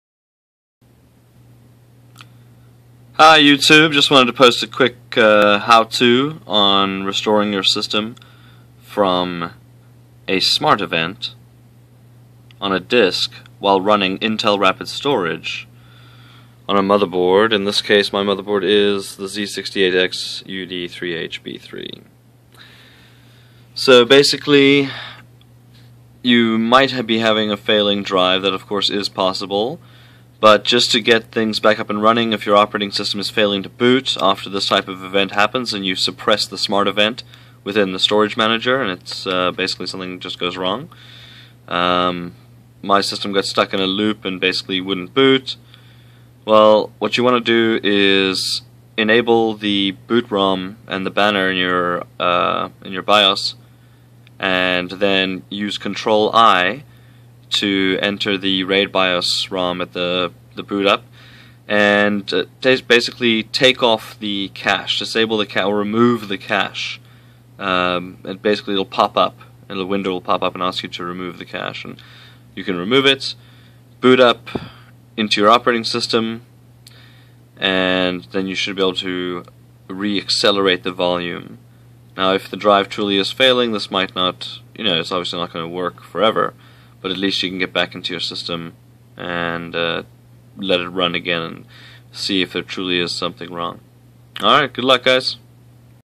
IRST SMART Event z68 Raid0 recovery
Restore disk acceleration after boot failure following a smart event. This was performed on a z68 chipset.